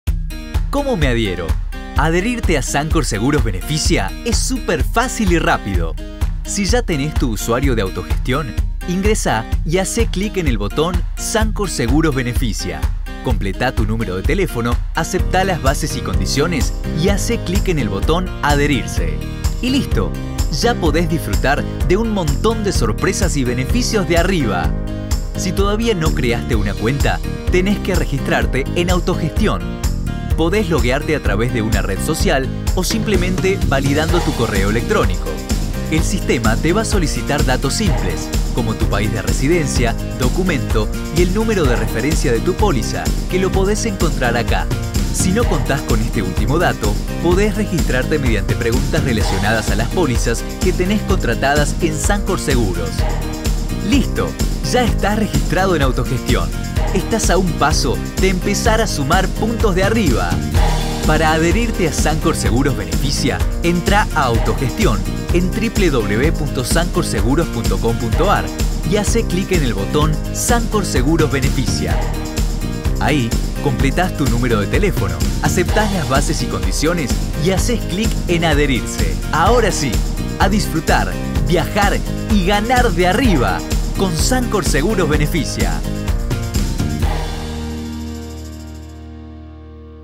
Sancor Seguros Beneficia: ¿Cómo me adhiero?
Conocé cómo es el proceso de adhesión a nuestro Programa de Beneficios exclusivo para nuestros clientes.